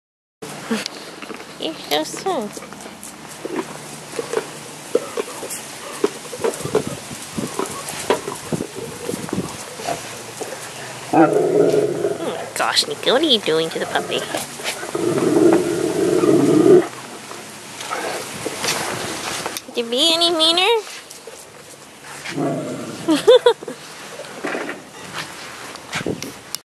Rocky and Cagney
Rocky plays with his puppy self.